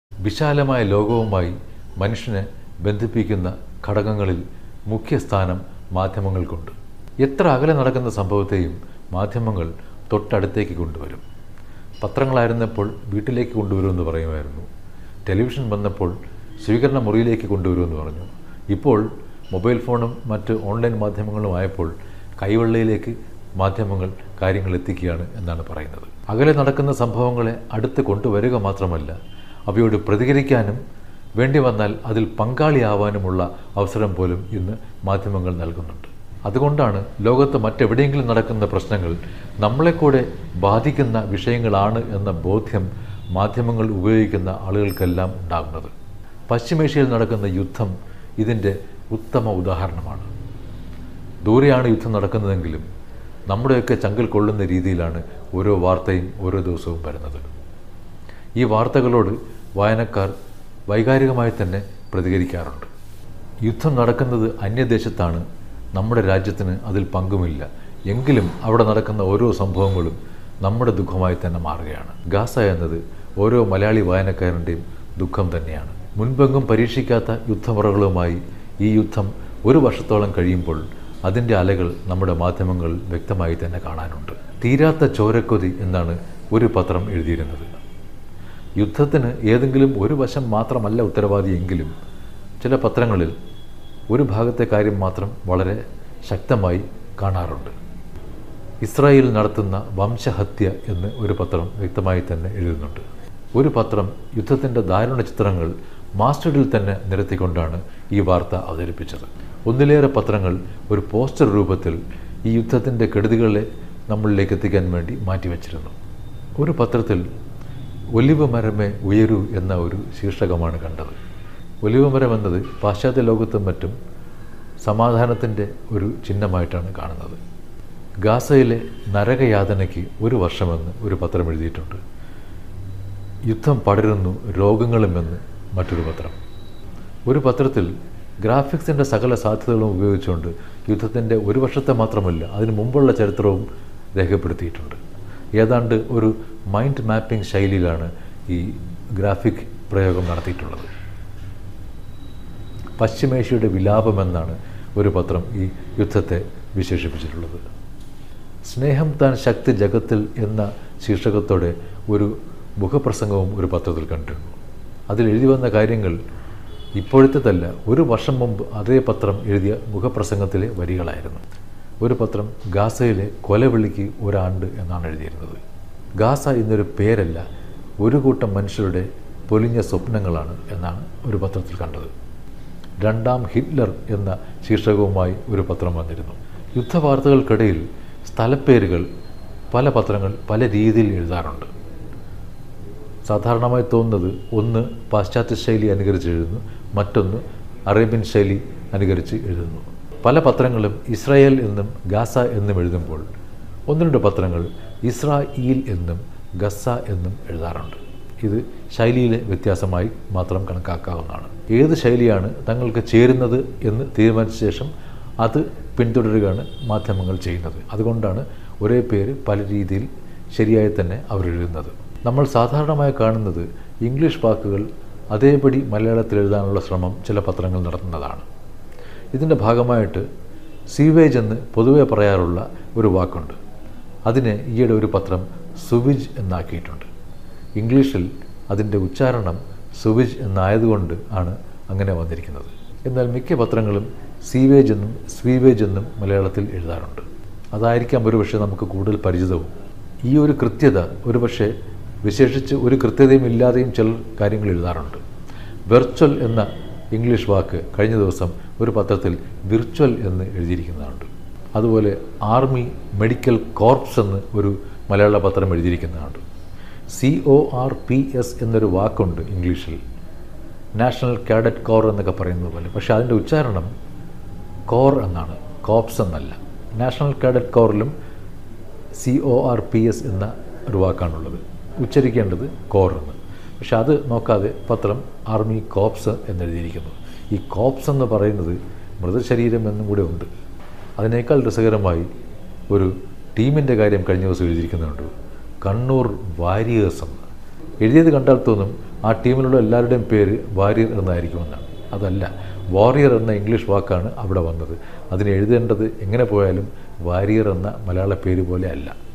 ഗാസയിൽ തുടരുന്ന യുദ്ധവും മാധ്യമങ്ങളും
ഒരു വർഷമായി തുടരുന്ന പശ്ചിമേഷ്യയിലെ യുദ്ധത്തെക്കുറിച്ച് മാധ്യമങ്ങൾ എന്തു പറയുന്നു എന്നാണ്
മാധ്യമജാലകത്തിലെ വാക്കിലെ ശരി ചർച്ച ചെയ്യുന്നത്.